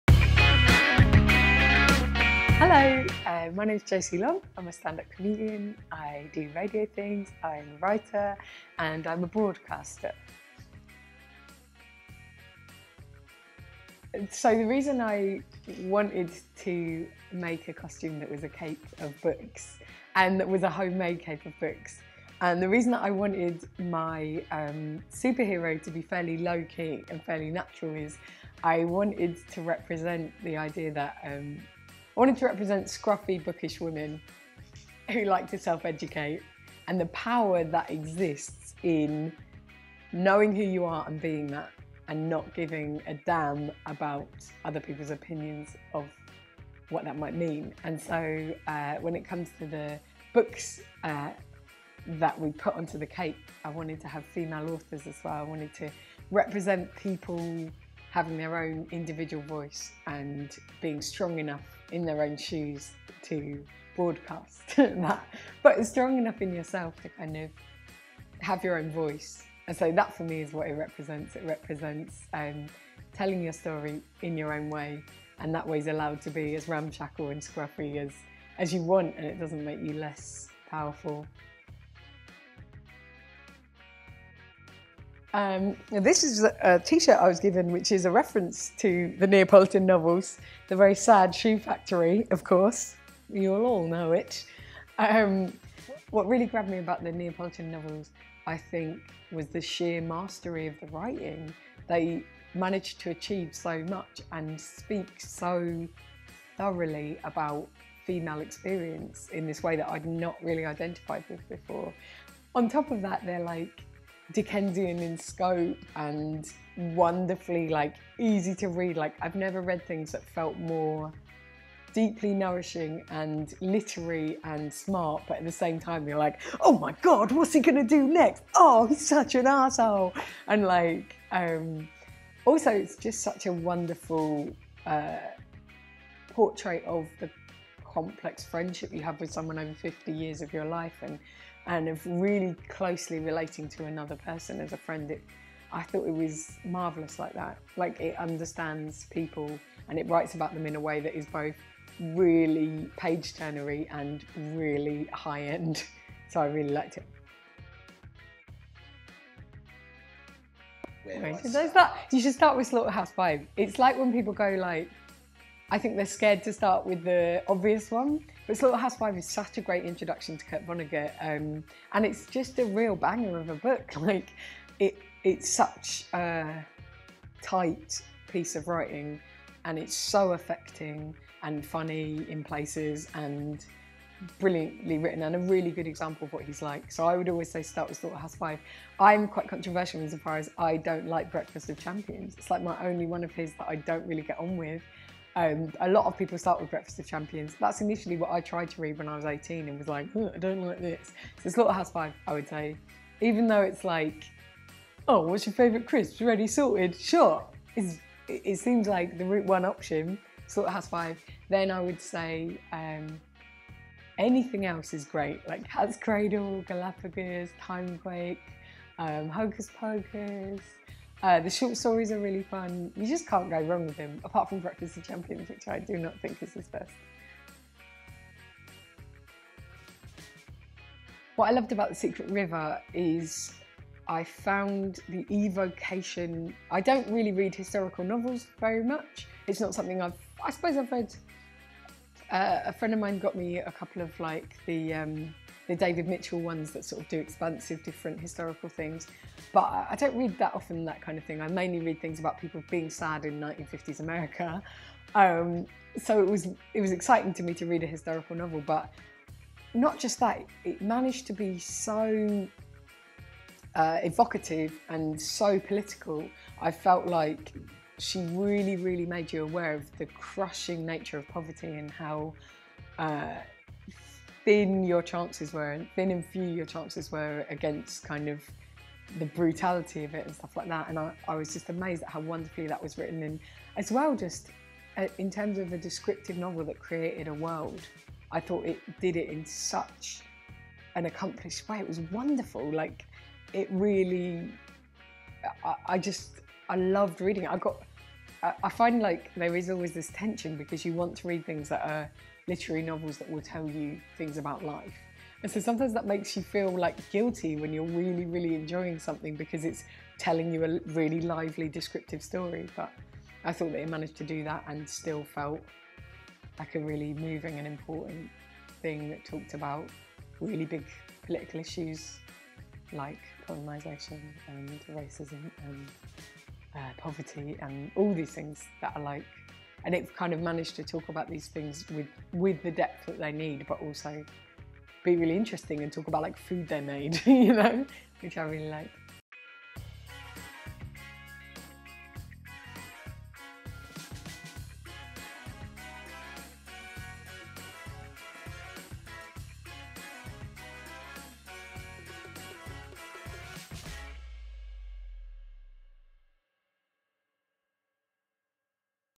Josie Long - Cosmic Superheroes
Cosmic Superheroes is a unique photographic celebration of the superpowers it has taken for women at the forefront of the worlds of Arts and Science to establish themselves as superheroes in their individual fields. The exhibition will be free to attend at Conway Hall in London from December 8th to January 31st.

Josie Long is a comedian, performer, writer & broadcaster

Josie is one of the country’s most critically acclaimed and well-loved comedians. She has been performing stand up since the age of 14 and has garnered numerous awards since, including the prestigious Best Newcomer at the Edinburgh Fringe.

Apart from touring the world with her stand up she also presents Short Cuts on the BBC and co-hosts the Book Shambles podcast with Robin Ince on The Cosmic Shambles Network.

New huge live show at the Royal Albert Hall in June 2018: Robin Ince and Chris Hadfield's Space Shambles.

Cosmic Superheroes produced and curated by Trent Burton and Melinda Burton

Photography by Timothy Ginn

Hair and Make Up by Emily Mickelburgh

Support from Conway Hall and the March for Science London